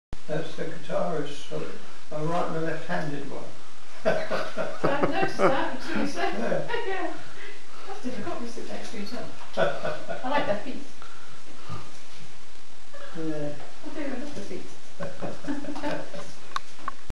franckmoore video03
Artist Franck Moore informally discussing his work